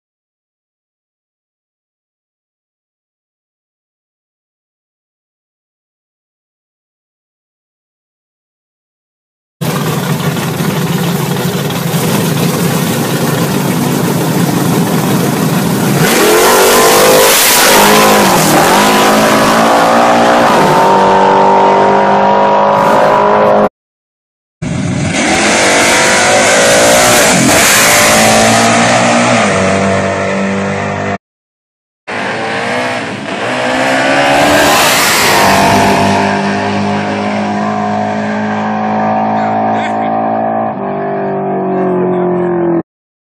Grey Boys vs S.R.G.
Grey boys nitrous small block chevy malibu vs S.R.G. nitrous small block chevy foxbody, heads up, nitrous to nitrous, $4100 up for grabs